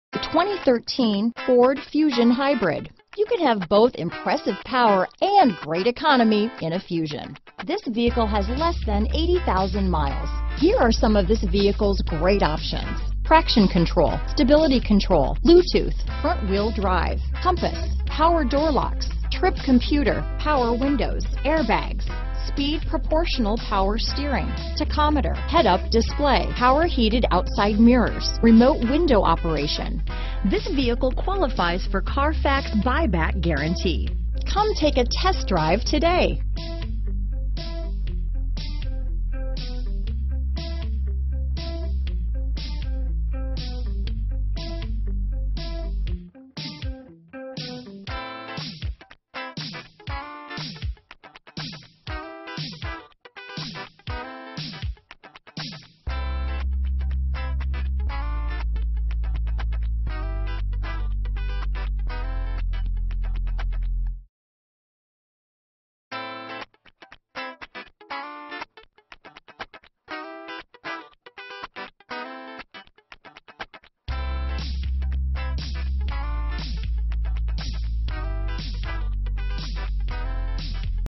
2013 Ford Fusion Hybrid Fall River, Dartmouth, New Bedford, Wareham, MA, Tiverton, RI T3317
Gray  2013 Ford Fusion Hybrid available in Fall River, Massachusetts at Prestige Auto Mart 1. Servicing the Dartmouth, New Bedford, Wareham, MA, Tiverton, RI area.

ATTENTION! Includes a CARFAX buyback guarantee!!! This tried-and-trued Hybrid Vehicle seeks the right match. Safety equipment includes: ABS, Traction control, Curtain airbags, Passenger Airbag, Signal mirrors - Turn signal in mirrors...Other features include: Bluetooth, Power door locks, Power windows, CVT Transmission, Climate control...